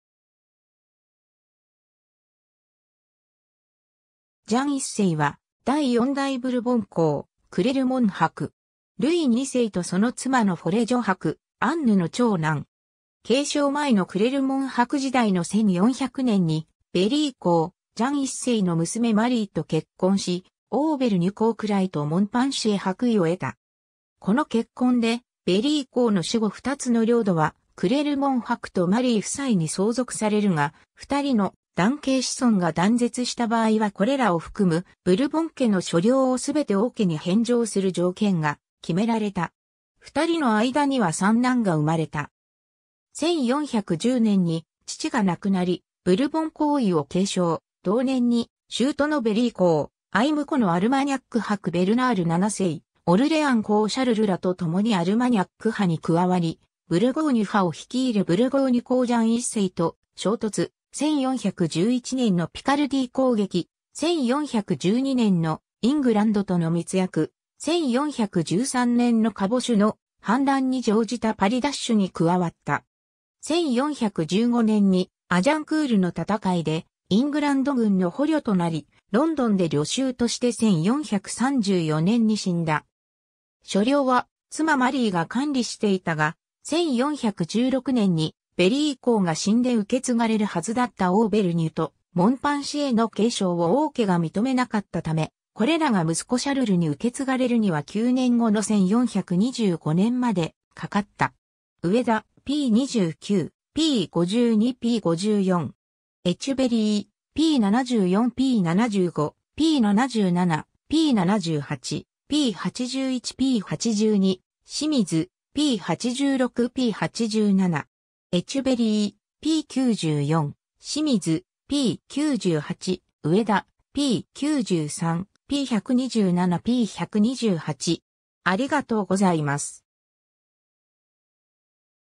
ジャン1世 (ブルボン公)
ジャン1世（Jean Ier, 1381年 - 1434年2月5日）は、第4代ブルボン公（在位：1410年 - 1434年）、クレルモン伯。
ルイ2世とその妻のフォレ女伯アンヌの長男。
 継承前のクレルモン伯時代の1400年にベリー公ジャン1世の娘マリーと結婚し、オーヴェルニュ公位とモンパンシエ伯位を得た。
この結婚でベリー公の死後2つの領土はクレルモン伯とマリー夫妻に相続されるが、2人の男系子孫が断絶した場合はこれらを含むブルボン家の所領を全て王家に返上する条件が決められた。
2人の間には3男が生まれた。
 1410年に父が亡くなりブルボン公位を継承、同年に舅のベリー公・相婿のアルマニャック伯ベルナール7世、オルレアン公シャルルらと共にアルマニャック派に加わり、ブルゴーニュ派を率いるブルゴーニュ公ジャン1世（無怖公）と衝突、1411年のピカルディー攻撃、1412年のイングランドとの密約、1413年のカボシュの反乱に乗じたパリ奪取に加わった。
 1415年にアジャンクールの戦いでイングランド軍の捕虜となり、ロンドンで虜囚として1434年に死んだ。
所領は妻マリーが管理していたが、1416年にベリー公が死んで受け継がれるはずだったオーヴェルニュとモンパンシエの継承を王家が認めなかったため、これらが息子シャルルに受け継がれるには9年後の1425年までかかった。
 ^ 上田、P29、P52 - P54。
 ^ エチュヴェリー、P74 - P75、P77、P78、P81 - P82、清水、P86 - P87。
 ^ エチュヴェリー、P94、清水、P98、上田、P93、P127 - P128。